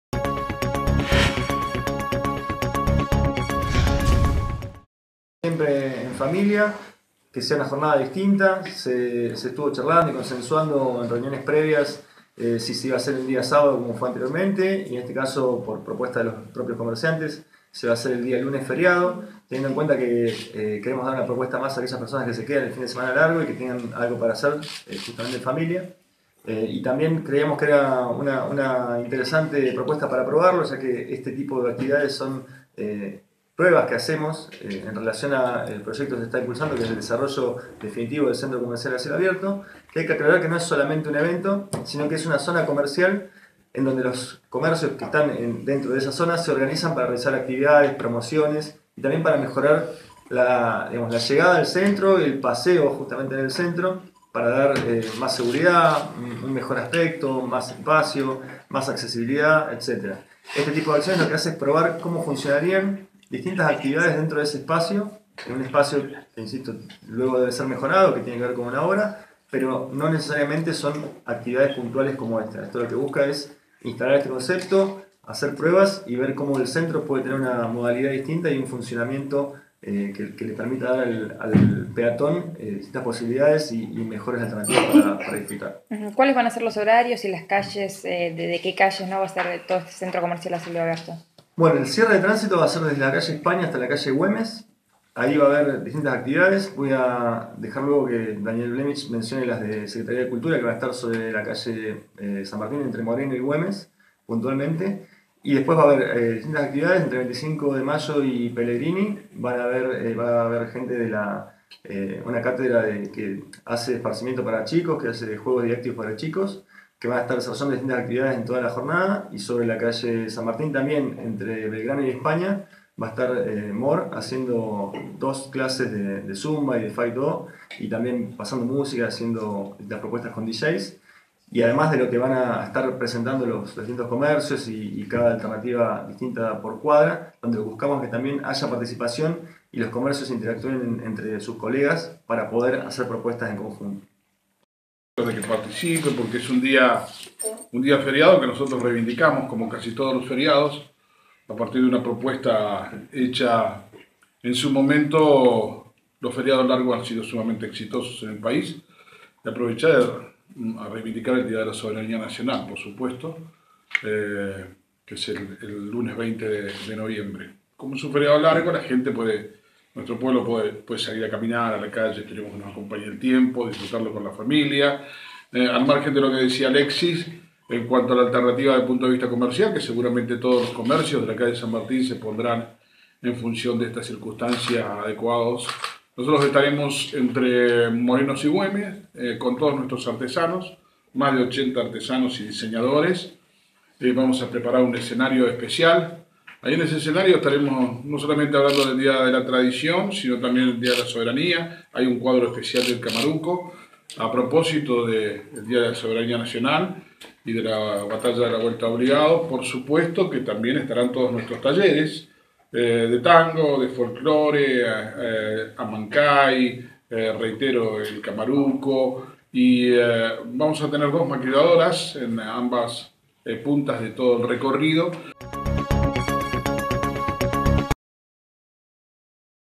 ESTE LUNES SE REALIZARÁ EL EVENTO DENOMINADO CENTRO COMERCIAL A CIELO ABIERTO.
Alexis Togel/Presidente de la Cámara de Comercio
Daniel Vleminchx/Secretario de Cultura